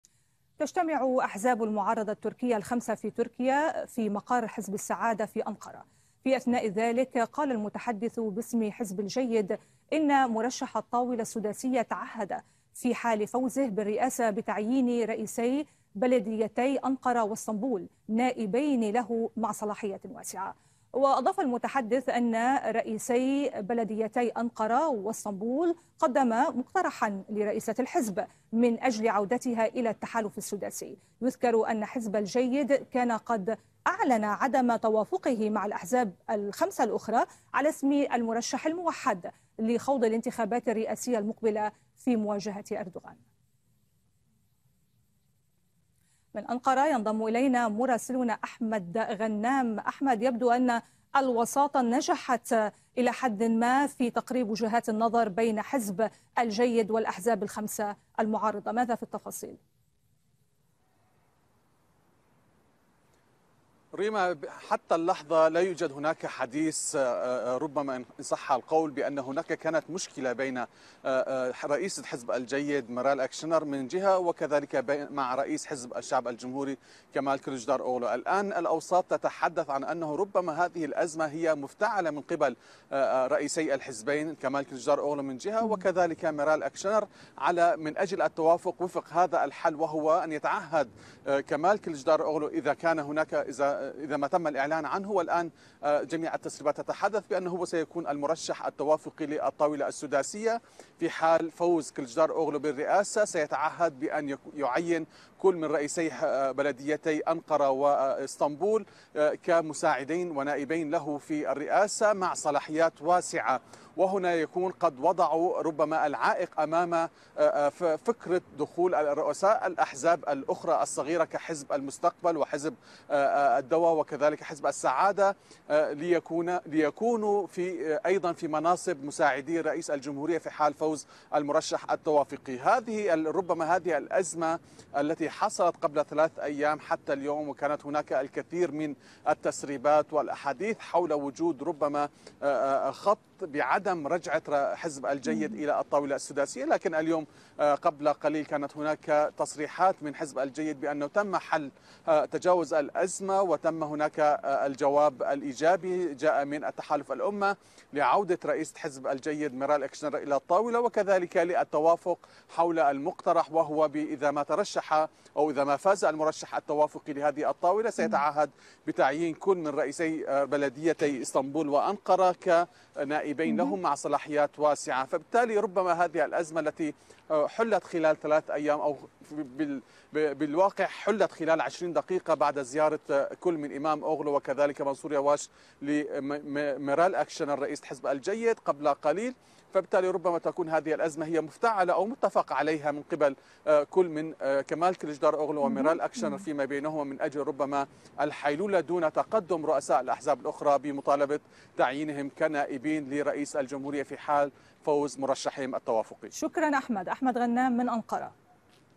تركيا .. أحزاب المعارضة تعلن اليوم عن مرشحها الموحّد
تجتمع اليوم أحزاب المعارضة لتسمية مرشحها في الانتخابات الرئاسية، وقال متحدث باسم حزب الجيد إن مرشح الطاولة السداسية يتعهد في حال فوزه بالرئاسة بتعيين رئيسي بلديتي أنقرة وإسطنبول نائبين له مع منحهما صلاحيات واسعة.